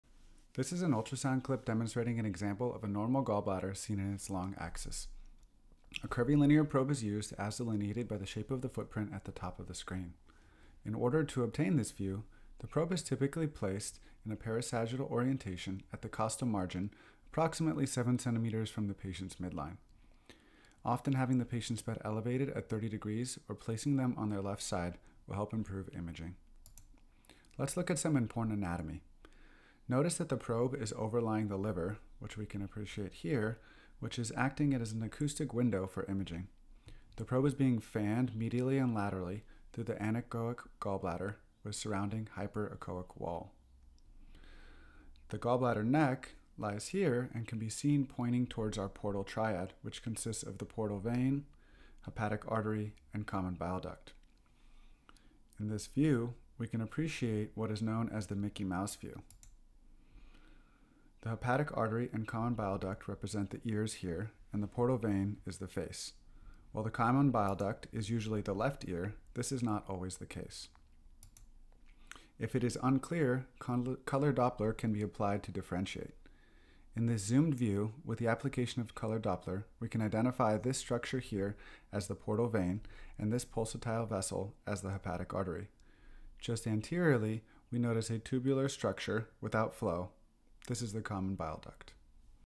Normal Gallbladder in Long Axis
In this video we discuss imaging of the gallbladder and identification of the portal triad.